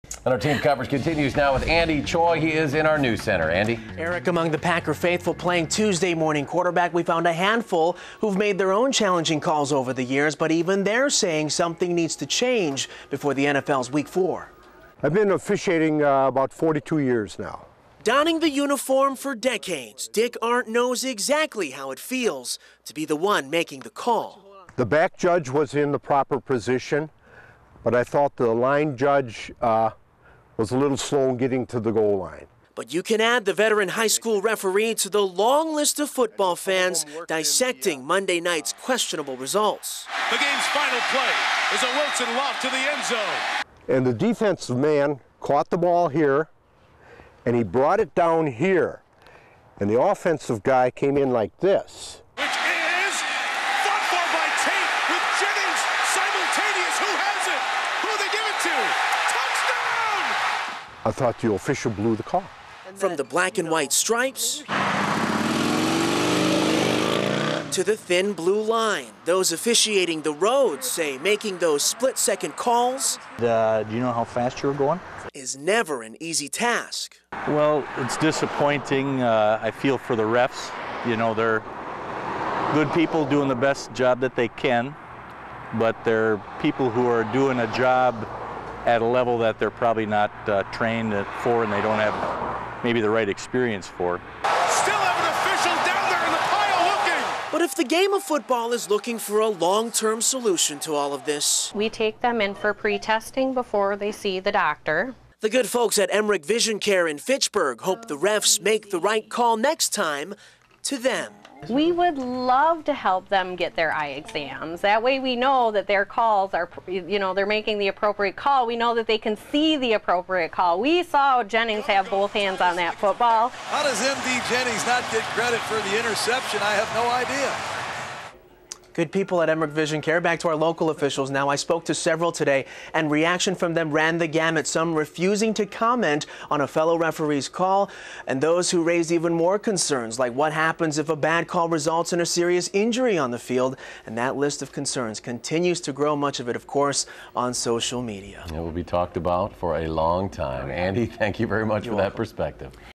Making The Call
Reaction to the play Green Bay Packers fans will discuss for years to come. Among those playing Tuesday morning QB, we found some who've made their own challenging calls over the years. With photojournalist Jim Rader.